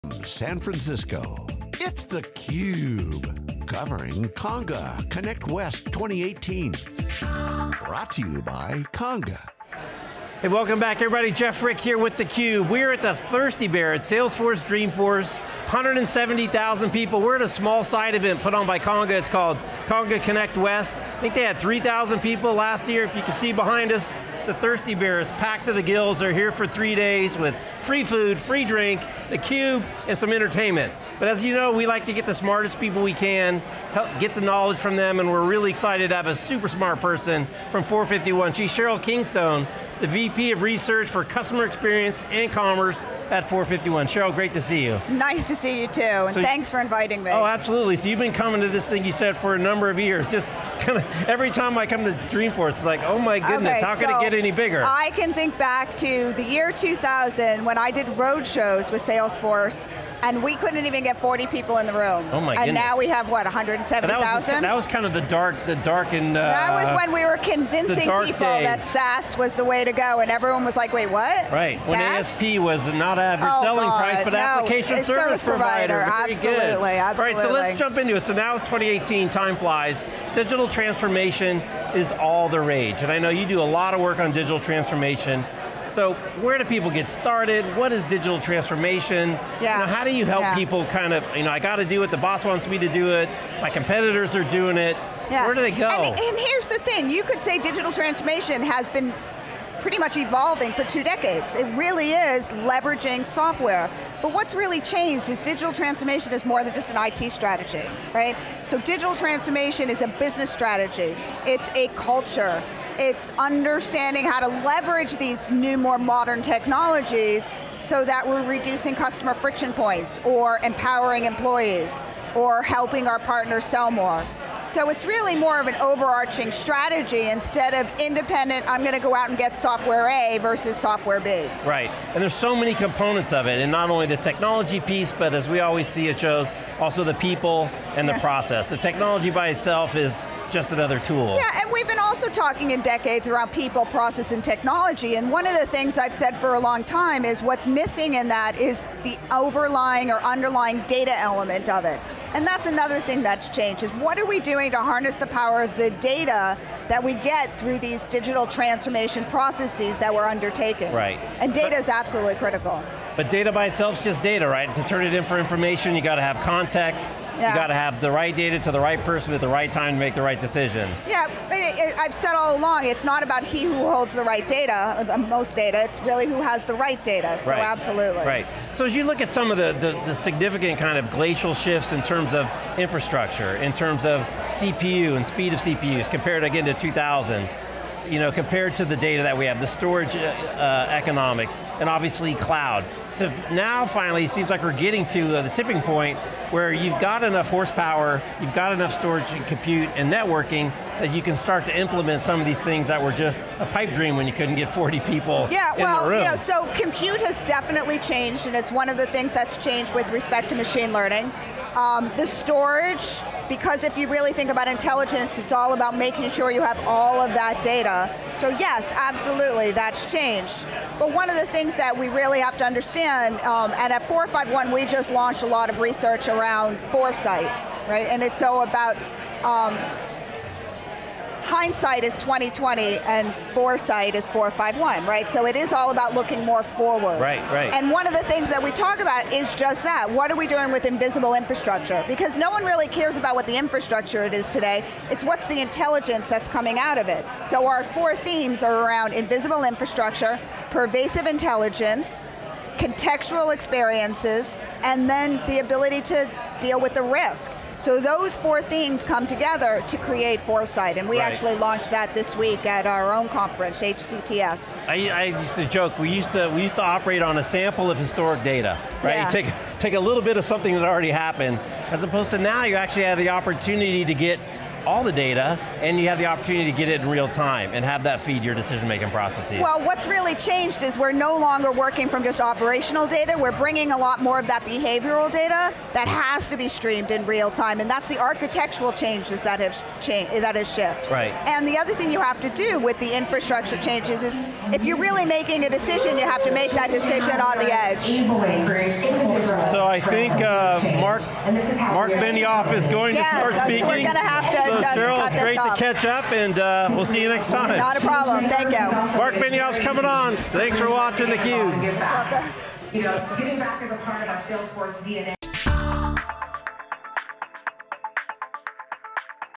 Sheryl Kingstone, 451 Research | Conga Connect West at Dreamforce 2018
Sheryl Kingstone, Research VP Customer Experience and Commerce, 451 Research sits with Jeff Frick for Conga Connect West at Dreamforce 2018 in San Francisco